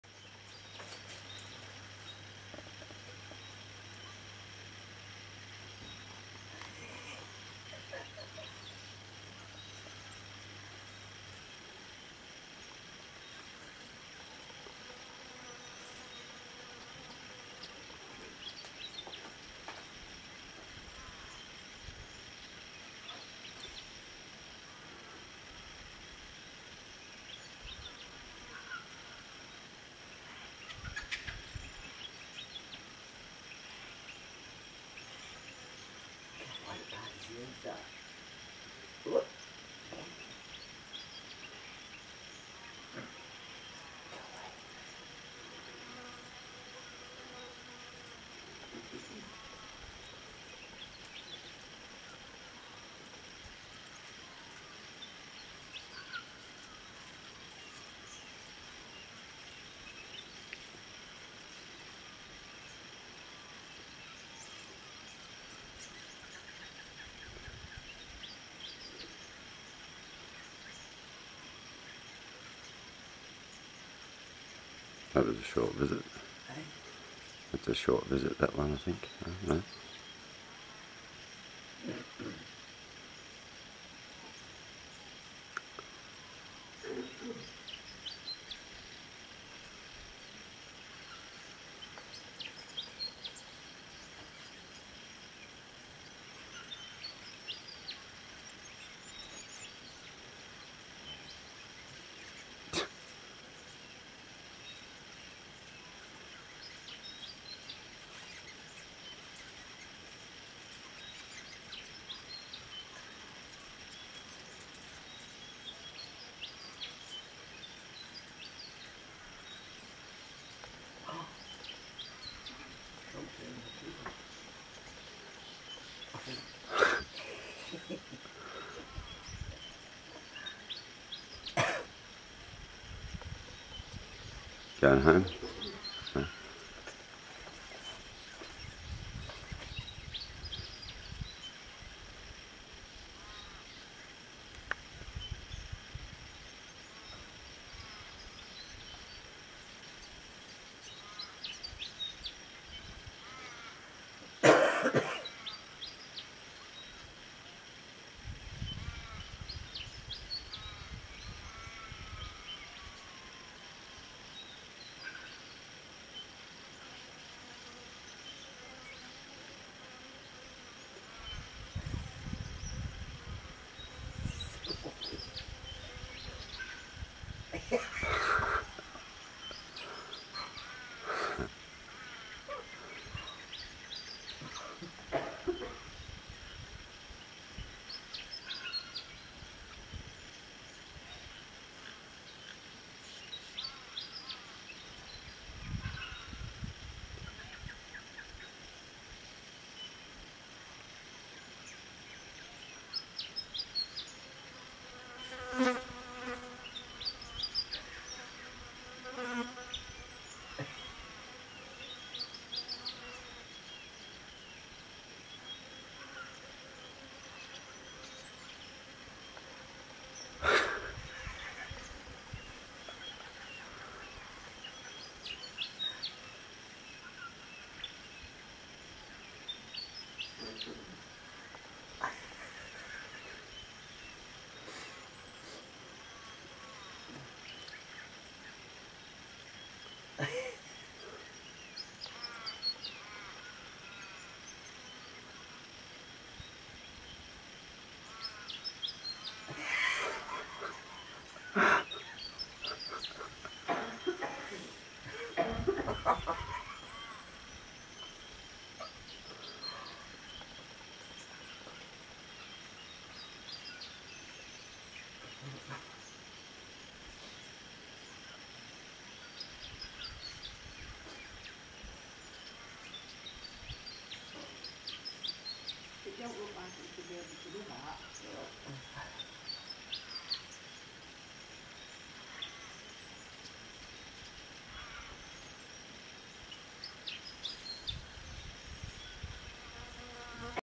joey learning to be a kangaroo
This video was shot early morning on thursday oct 25. We were staying at The Wilderness Resort in Walpole, Western Australia. About 30 wild kangaroos were feeding in the paddock by our chalet. Watching this young joey and his mother was just magic.
widgie48.